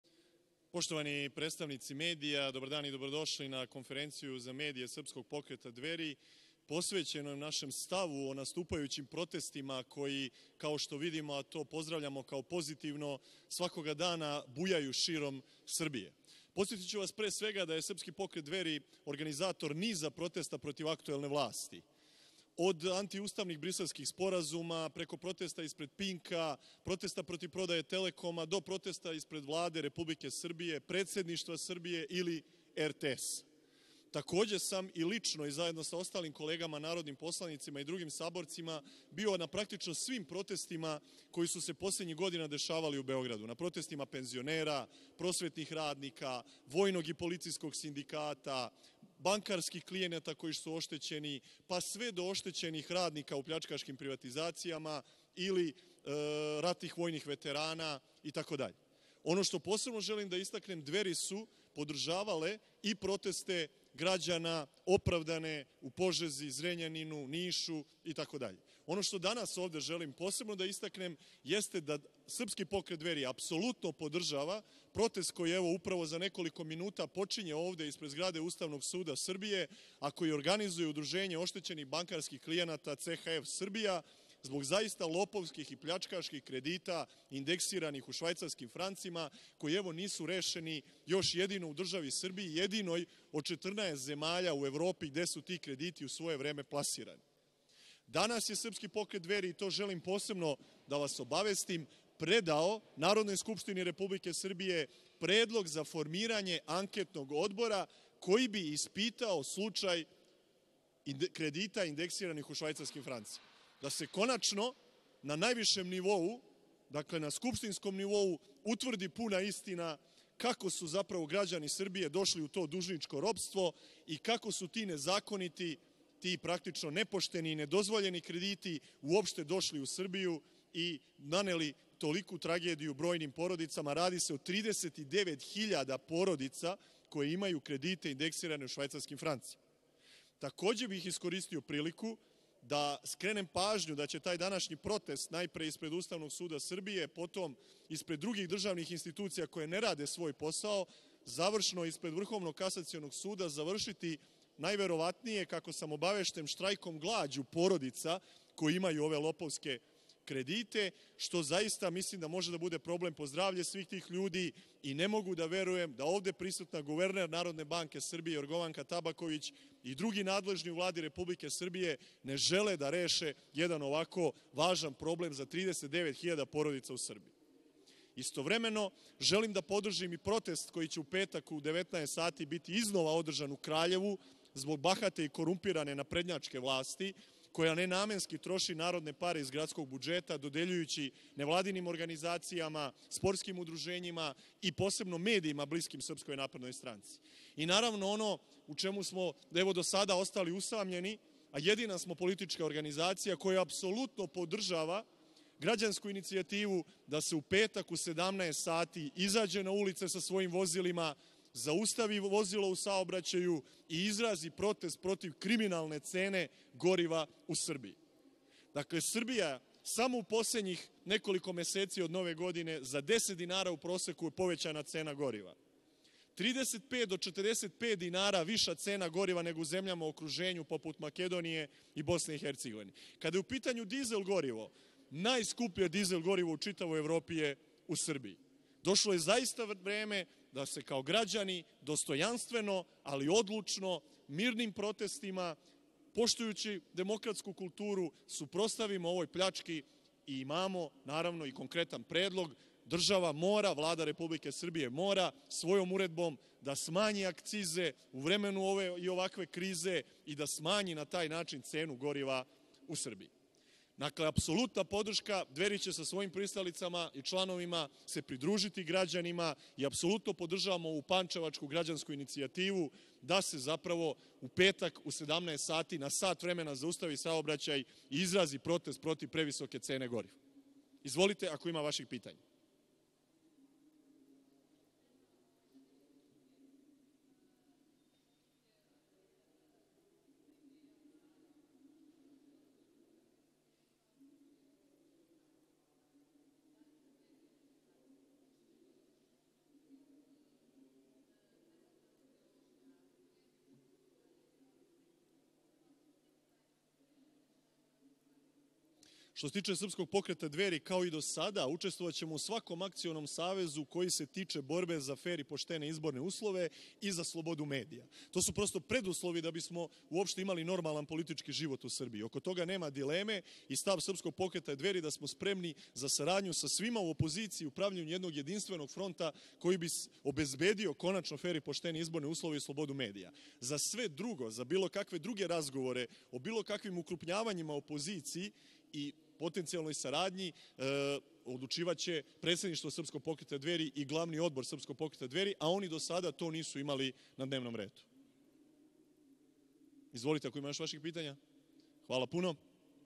Бошко Обрадовић: Утврдити одговорност због ситације са кредитима у швајцарцима
Председник Двери истакао је да треба утврдити ко је одговоран за нерешавање питања кредита у швајцарским францима и рекао да након конференције посланици Двери иду испред Уставног суда да се придруже протесту Удружења ЦХФ Србија.

КЗН народног посланика Бошка Обрадовића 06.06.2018.